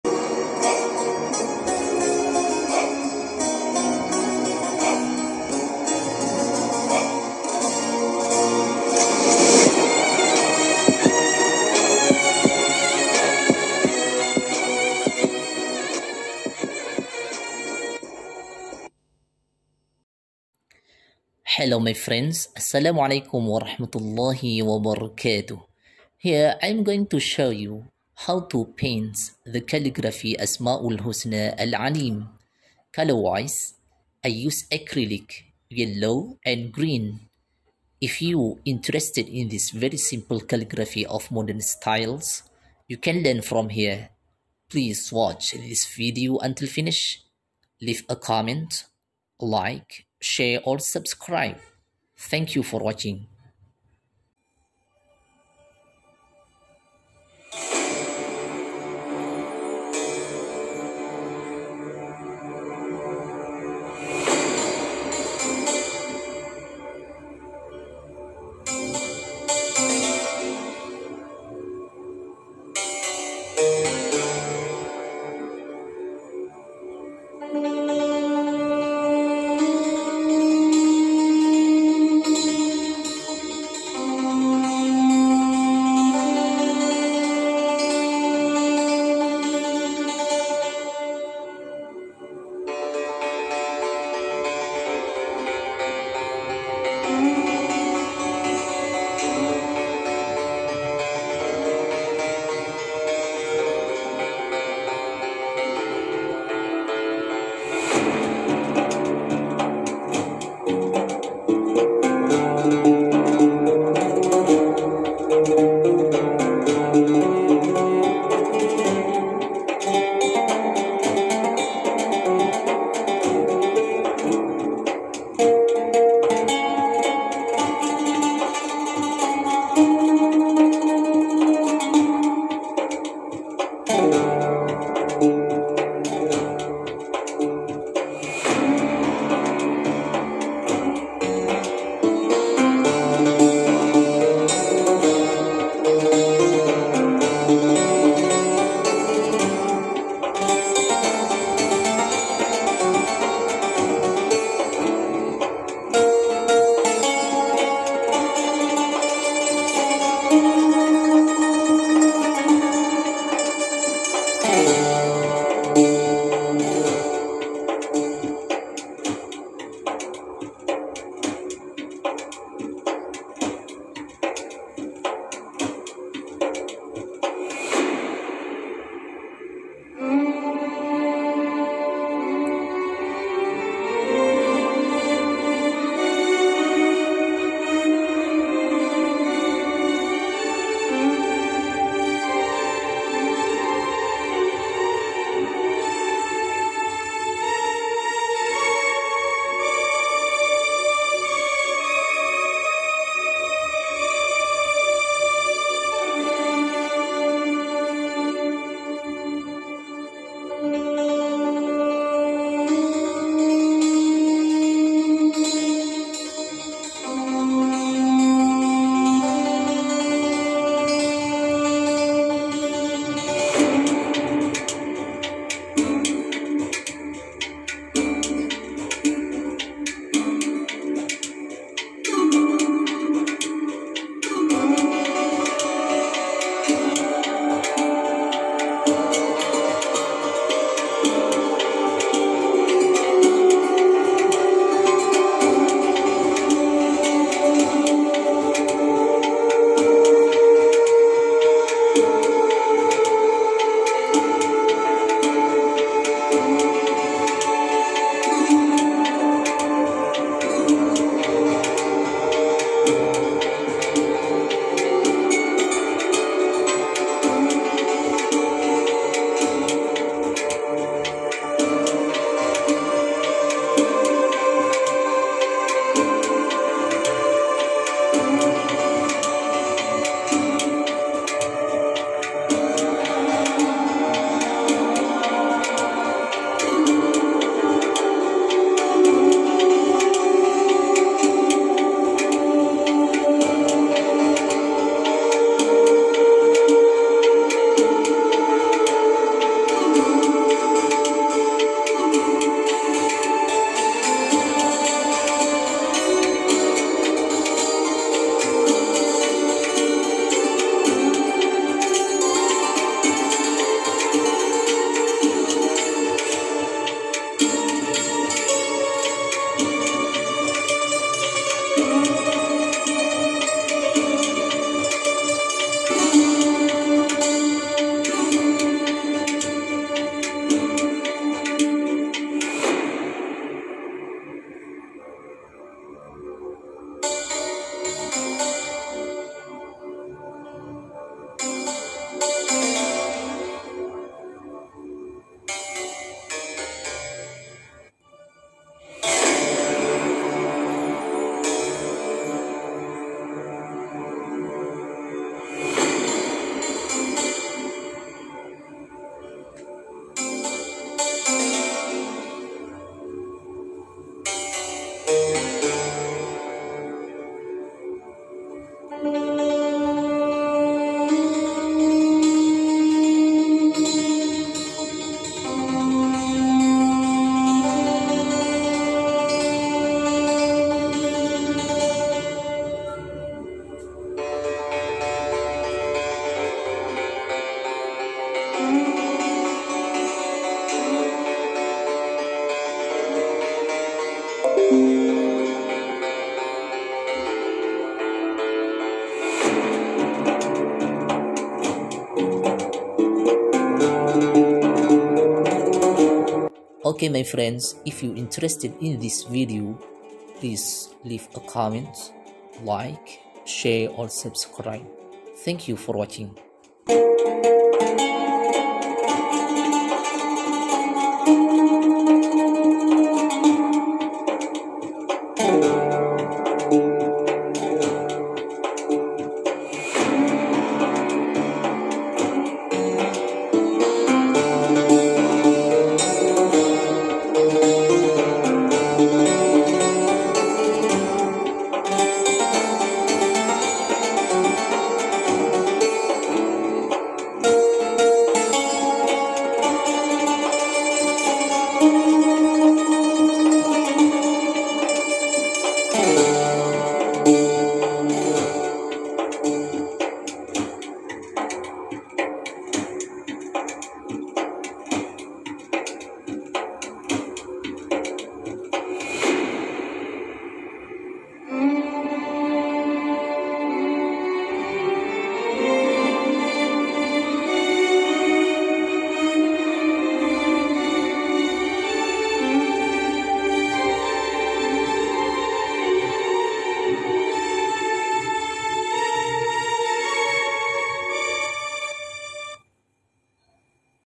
How To Create Arabic Calligraphy Asmaul Husna (Al-'Alim)?asmaulhusna
Hello my friends here I’m going to show you how to paint the calligraphy Asmaul Husna Al-'Alim. Colour wise, I use acrylic Yellow, and Green.

If you are interested in this very simple calligraphy of modern styles, you can learn from here.